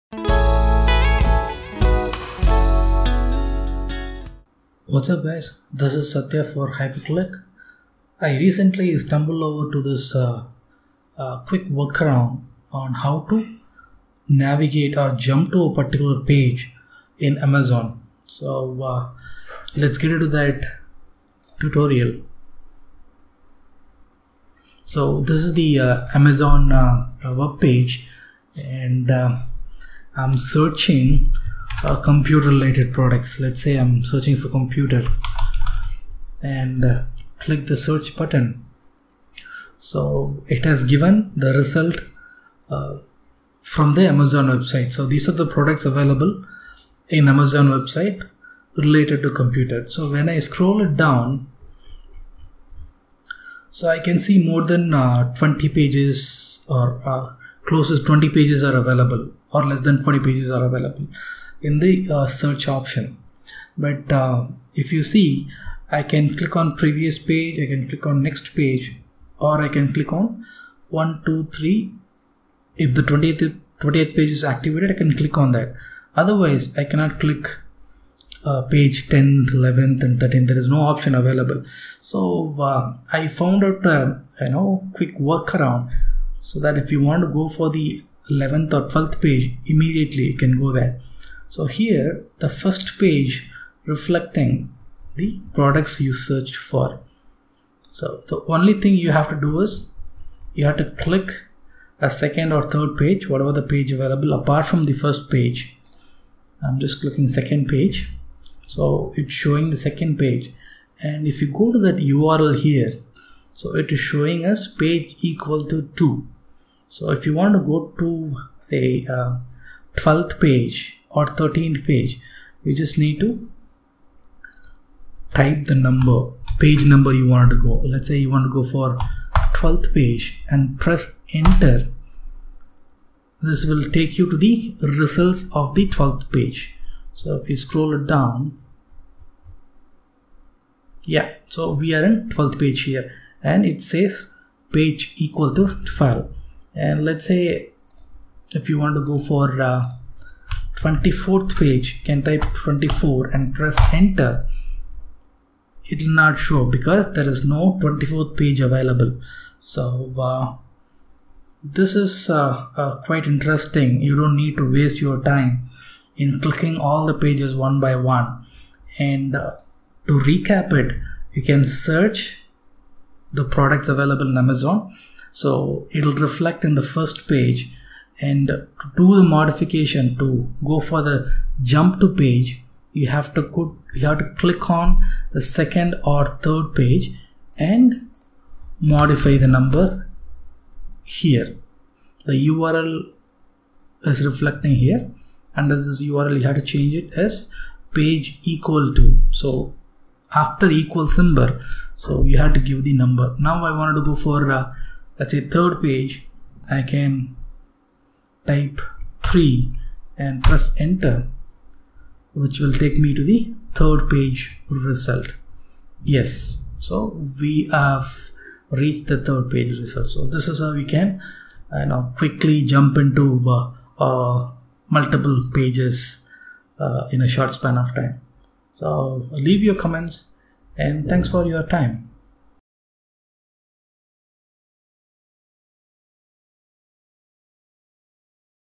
Amazon Search Page Jump
This video talks about the steps to jump between pages in Amazon search result

Jump to search pages using page number